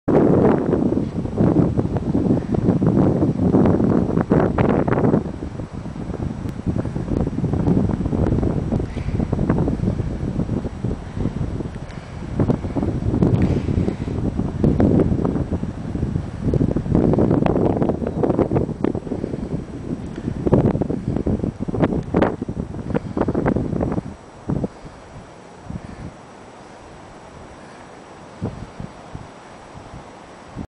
11 JUN 2009 BELGIAN COAST WESTENDE LOMBARDSIJDE SANERING CAMPING CRISTAL PALACE -
MOVIE TAVERNE CRISTAL PALACE AAN IDYLLELAAN HELEMAAL OPGERUIMD